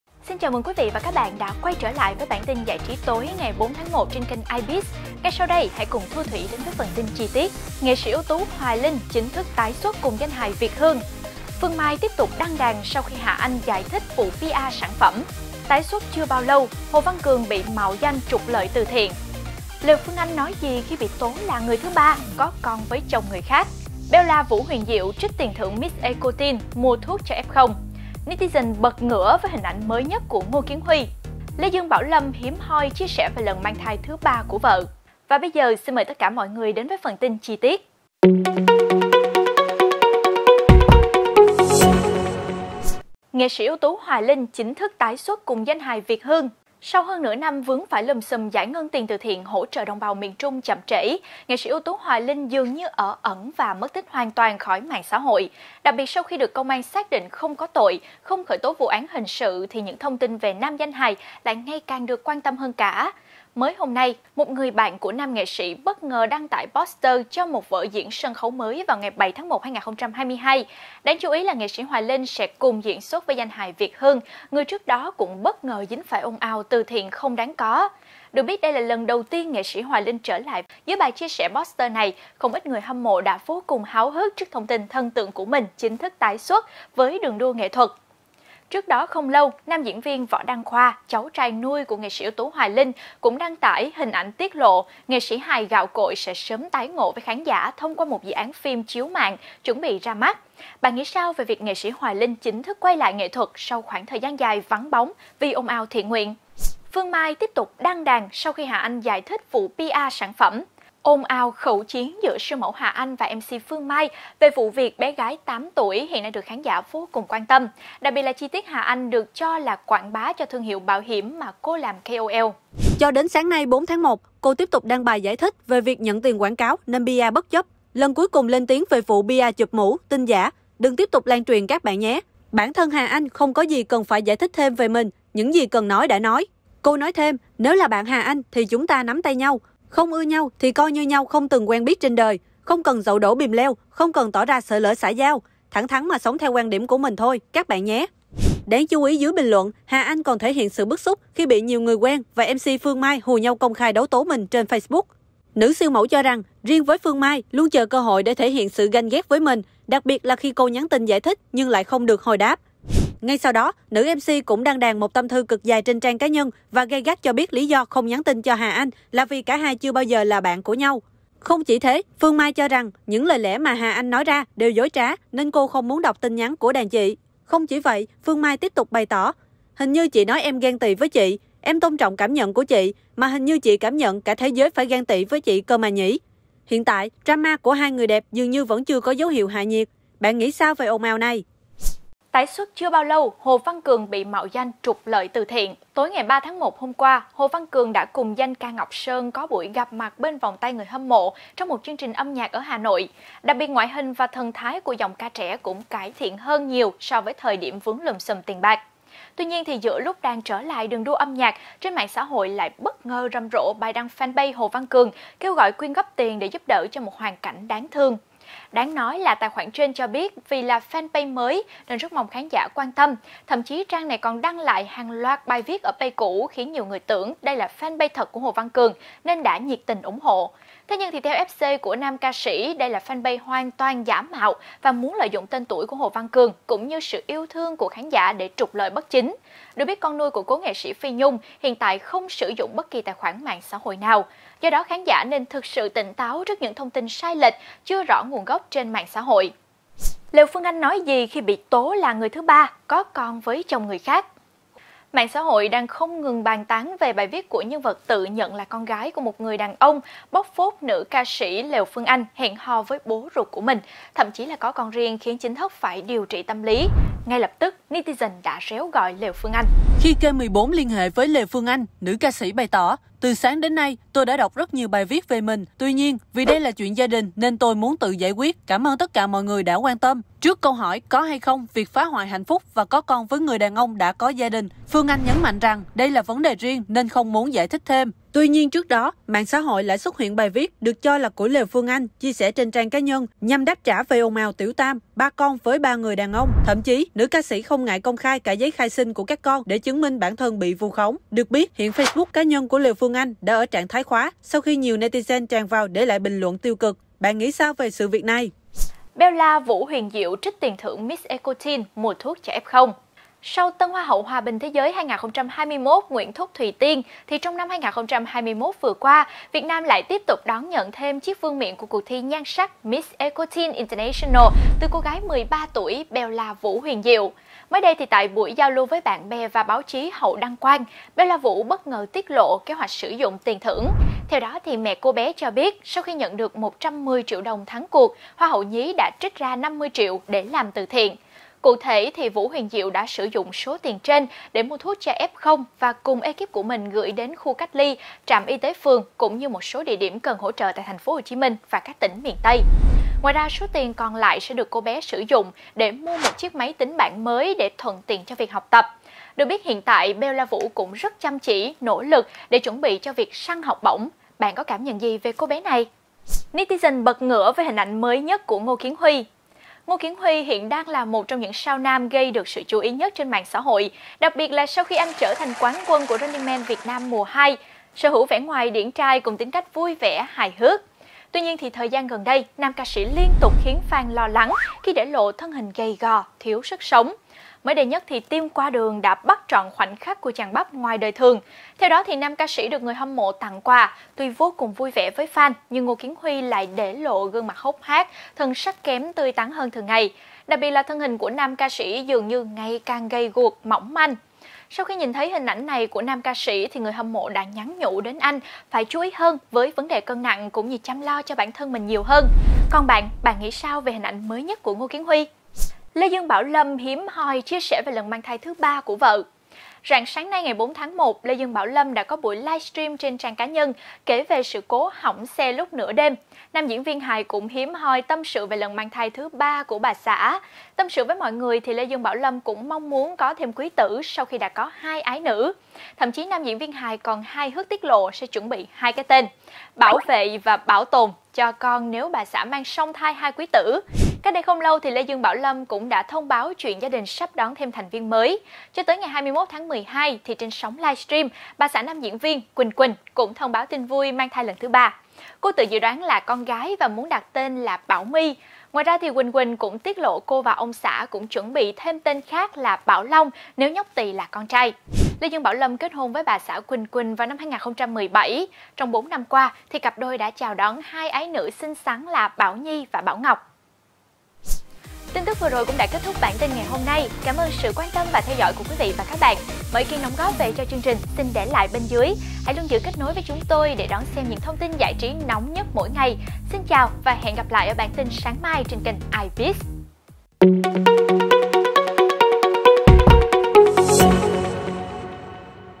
NSUT Hoài Linh chính thức trở lại sân khấu; Hồ Văn Cường kêu gọi quyên góp từ thiện? | Ibiz
NSUT Hoài Linh chính thức ‘tái xuất’ cùng danh hài Việt Hương
Phương Mai tiếp tục đăng đàn sau khi hà anh giải thích vụ PR sản phẩm
Tái xuất chưa bao lâu, Hồ Văn Cường bị mạo danh trục lợi từ thiện
Lều Phương Anh nói gì khi bị tố là người thứ 3, có con với chồng người khác?
Bella Vũ Huyền Diệu trích tiền thưởng Miss Eco Teen mua thuốc cho F0
Netizen ‘bật ngửa’ với hình ảnh mới nhất của Ngô Kiến Huy
Lê Dương Bảo Lâm hiếm hoi chia sẻ về lần mang thai thứ 3 của vợ